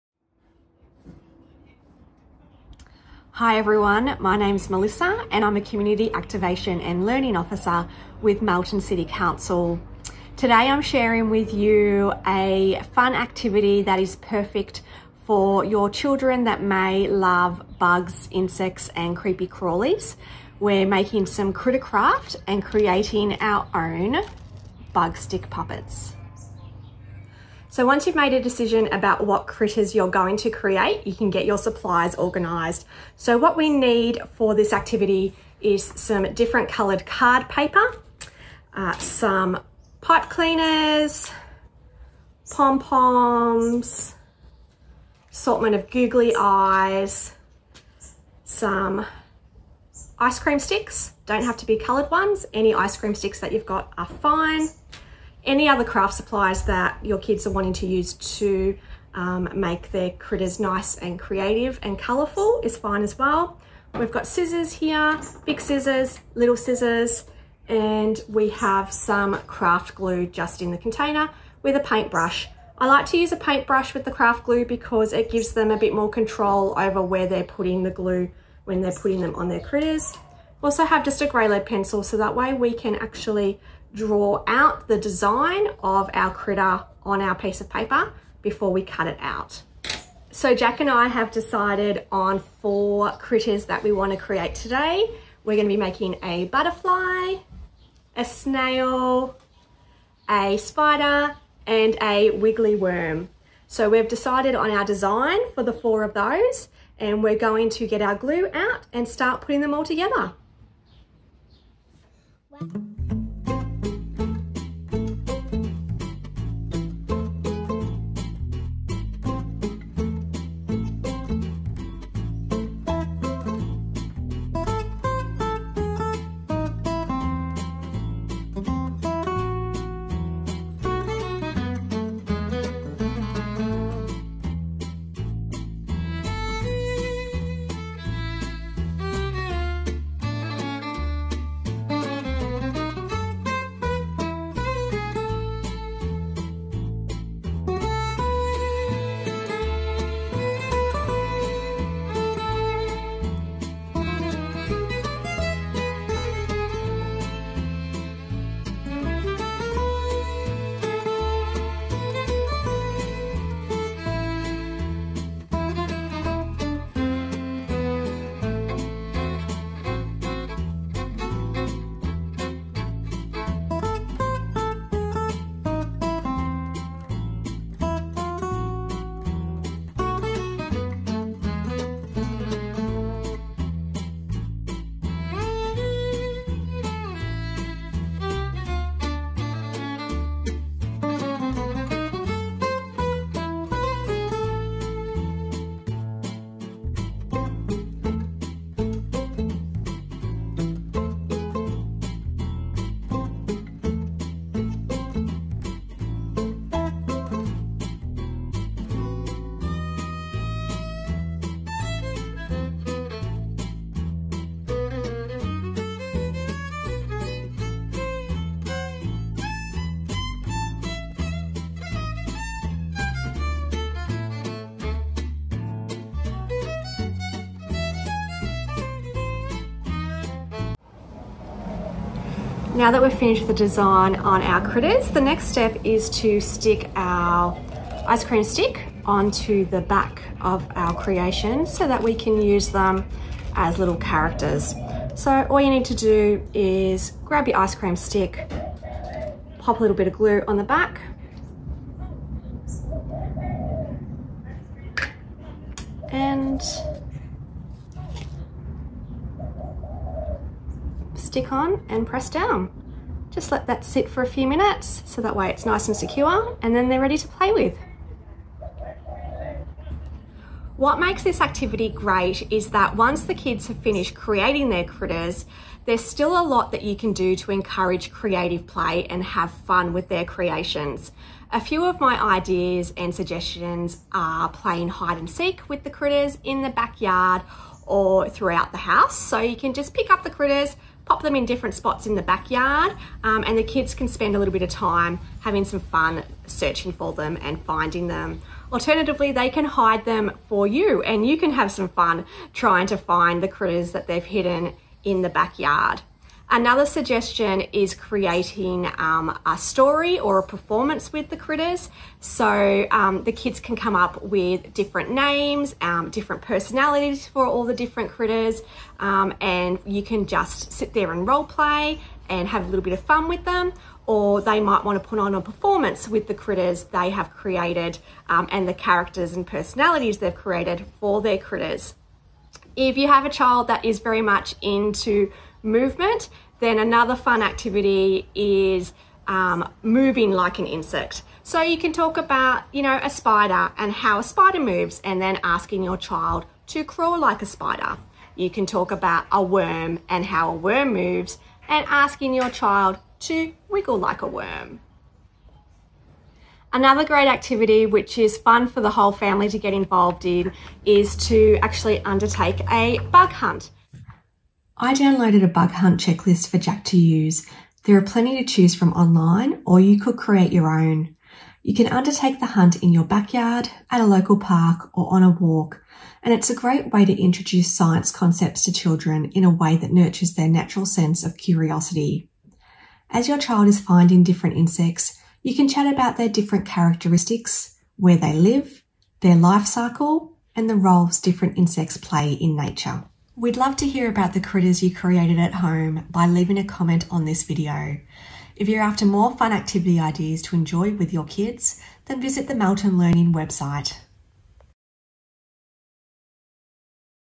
Critter Craft: DIY Bug Stick Puppets
Welcome back to the Melton City Council Learning Directory YouTube Channel!

If your kids love creepy crawlies, join Council officer Melissa for this fun and easy craft activity! Keep the kids entertained while encouraging them to head outside to explore and discover the small but mighty creatures that live in their own backyards.

In this video we will show you how to create bug stick puppets using craft supplies commonly found at home. This is a great activity to encourage imagination and active learning as children can select their favourite bugs to design and create.

The fun doesn’t stop there, Melissa will share some great bug themed activity ideas that will put your stick puppets to good use.

This activity is designed for pre-school aged children but can be modified for older kids as well.

SUGGESTED EQUIPMENT:
o Pipe cleaners
o Pom poms (assorted sizes)
o Googly eyes
o Ice-cream sticks
o Thick card paper – assorted colours
o Craft glue
o Coloured pencils/ crayons
o Glitter